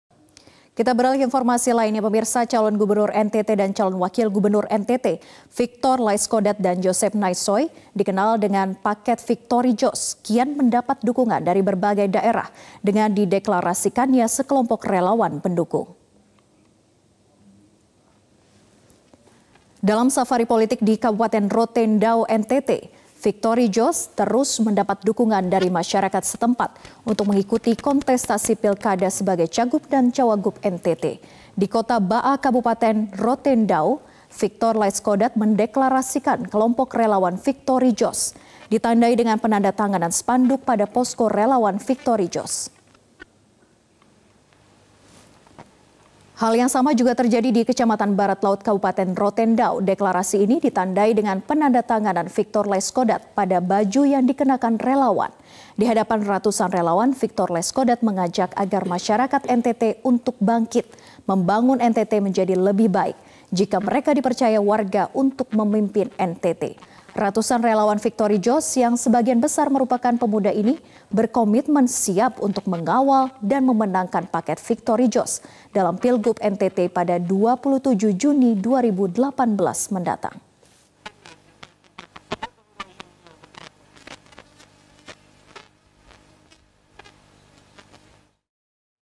Victory Joss Terus Mendapat Dukungan Masyarakat
Calon gubernur NTT dan calon wakil gubernur NTT Viktor Laiskodat dan Josef Nae Soi (Victory Joss) kian mendapat dukungan dari berbagai daerah dengan dideklarasikannya sekelompok relawan pendukung.